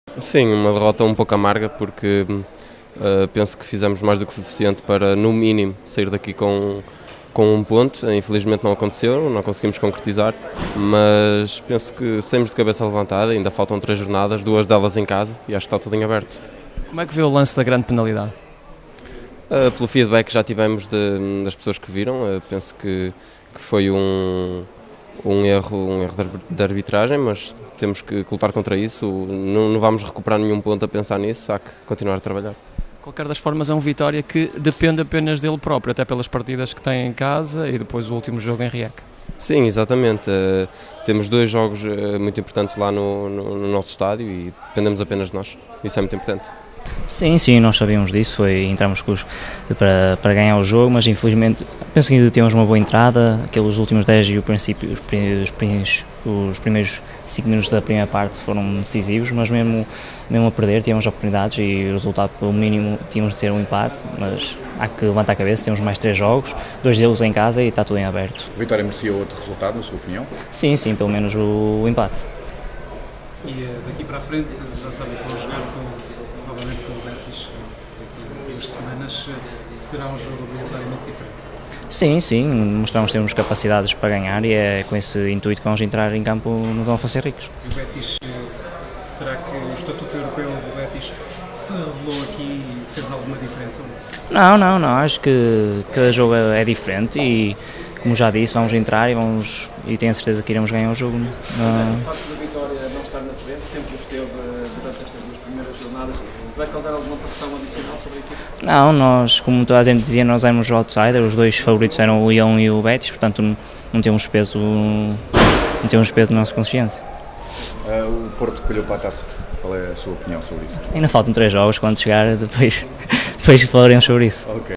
Jogadores do Vitória inconformados com derrota em Sevilha
| Paulo Oliveira e André André saíram do Benito Villamarim inconformados com a derrota com o Bétis de Sevilha.

Para os dois jogadores, o Vitória terá, dentro de 15 dias, oportunidade de rectificar, vencendo os espanhóis em Guimarães.

Paulo Oliveira lamentou ainda a decisão do árbitro grego, que não assinalou grande penalidade sobre Malonga, reiterando que a sua equipa "fez o suficiente para sair, no mínimo, com um ponto".

André André comunga da mesma opinião e ressalva que "ainda há três jogos, dois deles em casa", estando por isso "tudo em aberto".